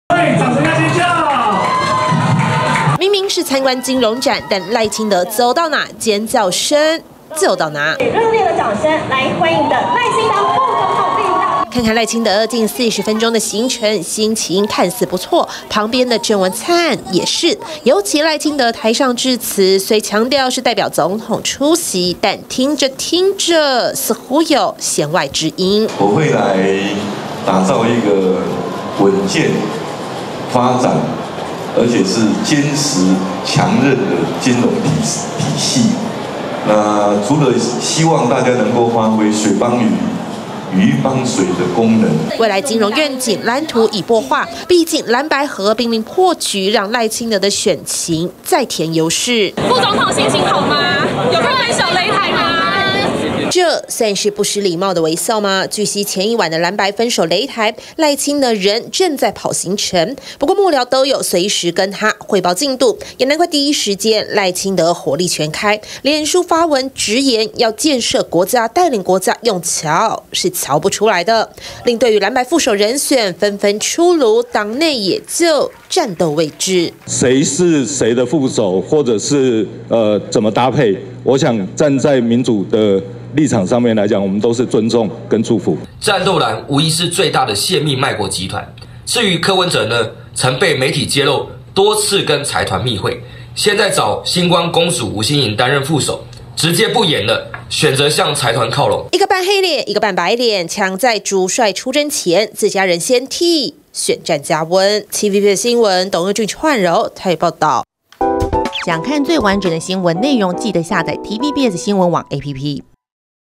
酸藍白合! 賴清德:建設國家是「喬」不出來的｜TVBS新聞
藍白合破局，也讓民進黨總統參選人賴清德的選情，再添優勢，據悉昨天一整天，賴清德跑了11個行程，並無空檔緊盯藍白合的分手擂台，不過幕僚是隨時彙報更新進度，也讓賴清德第一時間大酸，國家建設用喬，是喬不出來的！